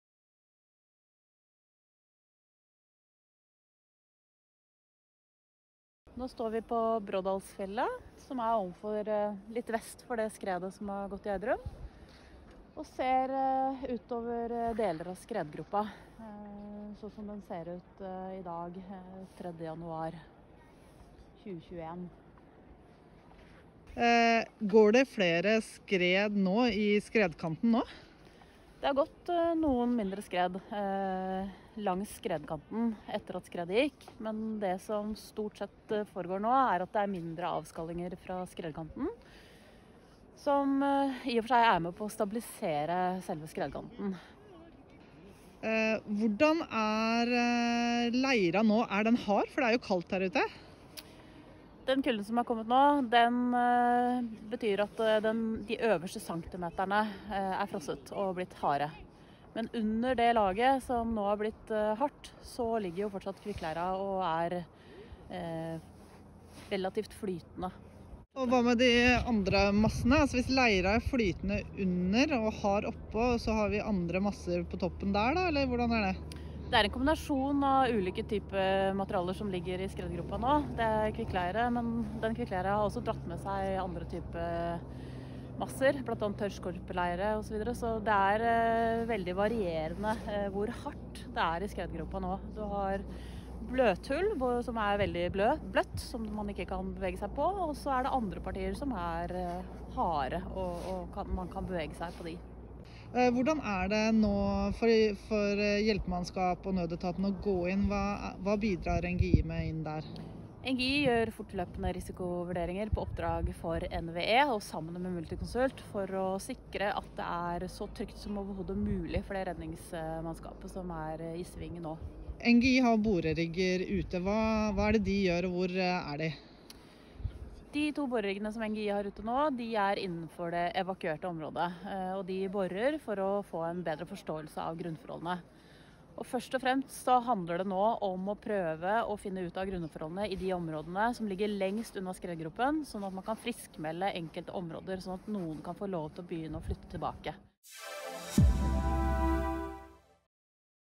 Skredet i Gjerdrum: Spørsmål og svar
Ingeniørgeolog og områdedirektør hos NGI, Guro Grøneng, svarer på spørsmål i forbindelse med skredet i Gjerdrum kommune 30. desember 2020.

English:

Engineering geologist and director of GeoMiljø at NGI, Guro Grøneng answers questions in connection with the quick clay landslide in Gjerdrum municipality on 30 December 2020.